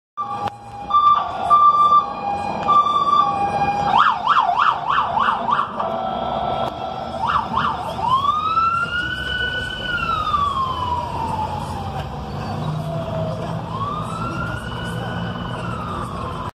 Ambulancia de Protección Civil Actopan respondiendo - Actopan fire department ambulance responding
Ambulancia Ram Promaster perteneciente a Protección Civil de Actopan Hidalgo respondiendo.

EN: Ram Promaster ambulance belonging to Actopan fire department responding.

Saludos - 👋